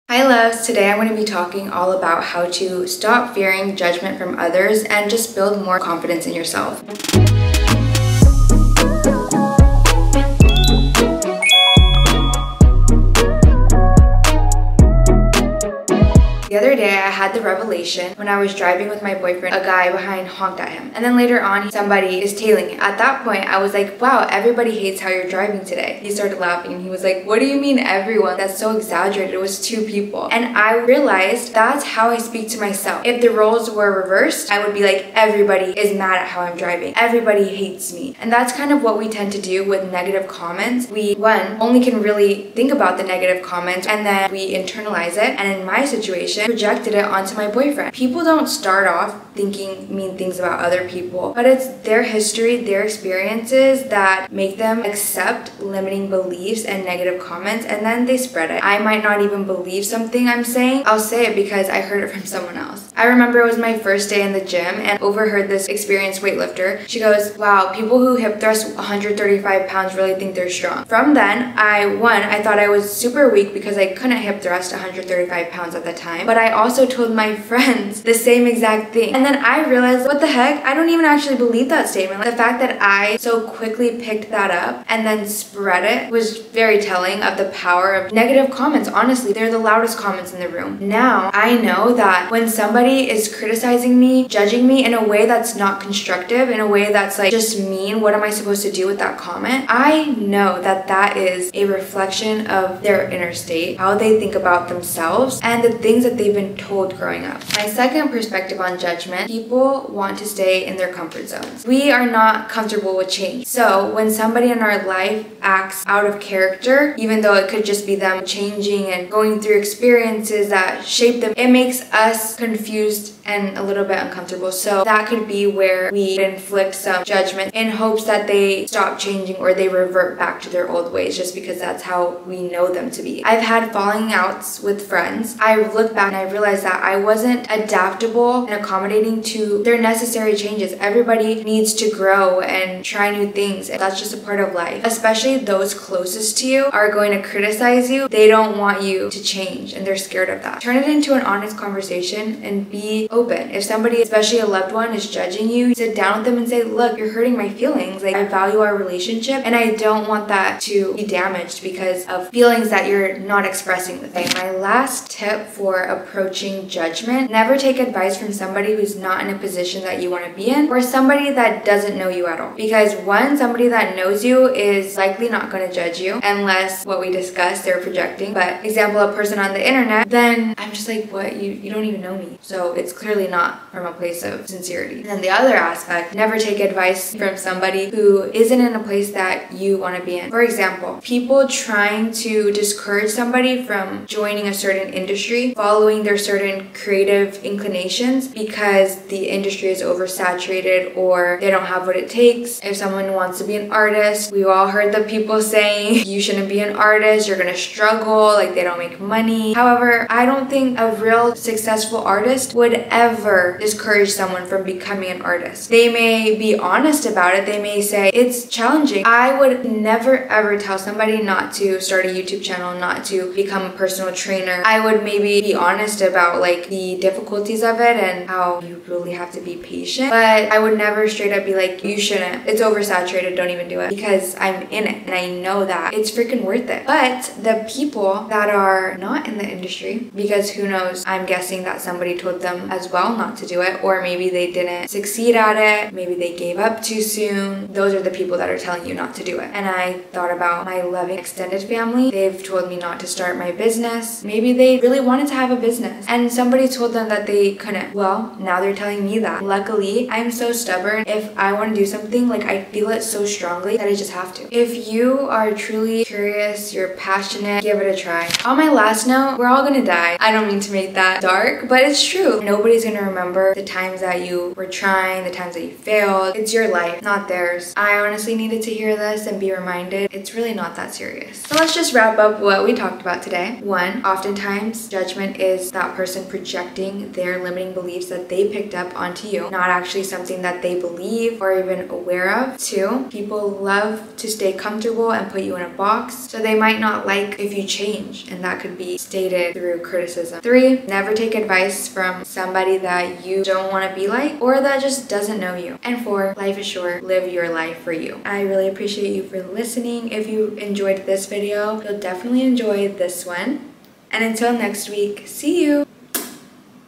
how to quickly stop worrying what people think (4 principles)
how to stop being a slave to the opinions of others 💪🏼 judgement is normal and unavoidable, but i hope this helps you realize that it's not that deep and it does not change your self worth. 🫶🏼